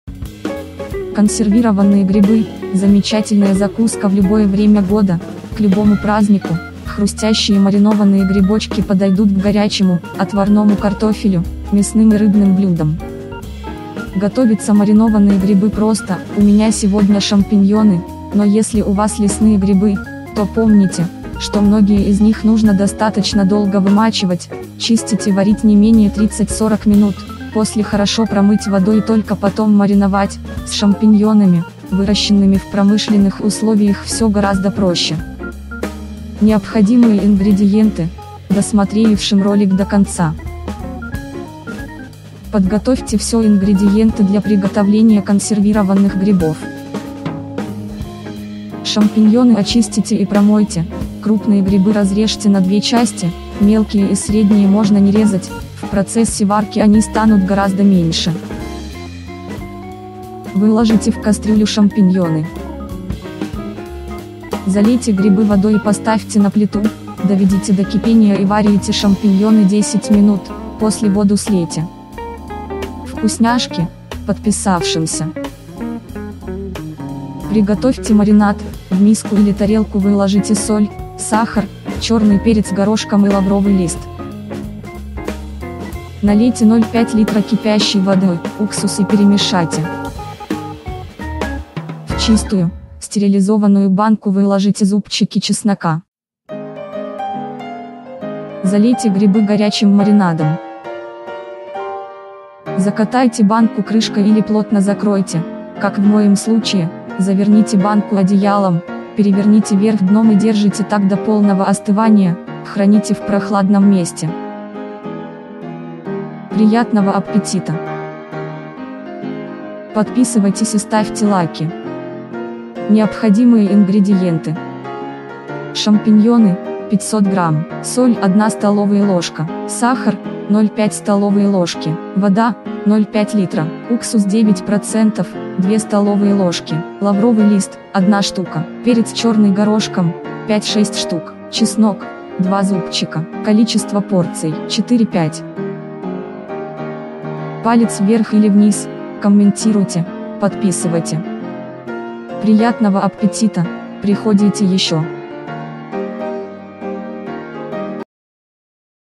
★ЭТО ВКУСНО!★ ГРИБЫ КОНСЕРВИРОВАННЫЕ ★
★Супервкусняшка● Консервированные грибы - замечательная закуска в любое время года, к любому празднику● Хрустящие маринованные грибочки подойдут к горячему, отварному картофелю, мясным и рыбным блюдам●